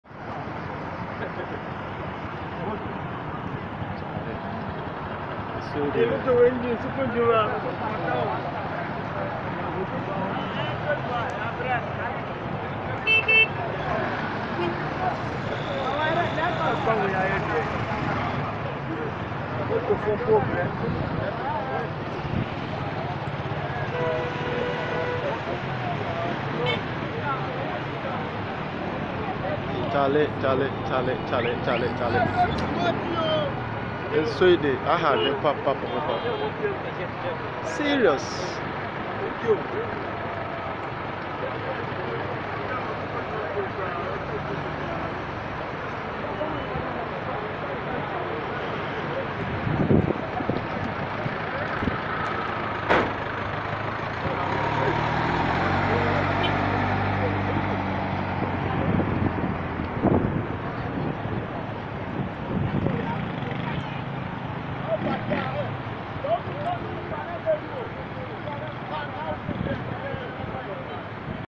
Accra-Tema Motorway floods.
After the downpour today October 28,2019, Accra Tema motorway was reduced to a flood scene just like in the movies.. Watch.